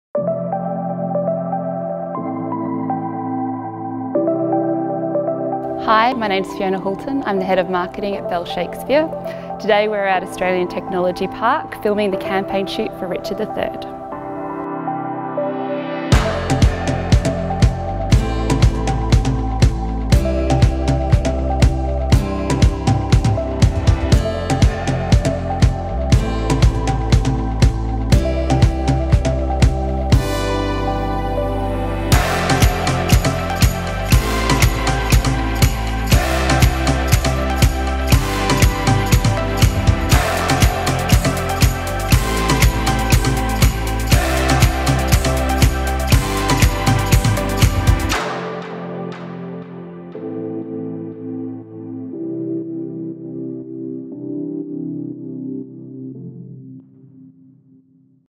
Behind the scenes on Richard 3 photo shoot
Join us behind the scenes as we shoot Kate Mulvany as Richard 3 for Bell Shakespeare 2017.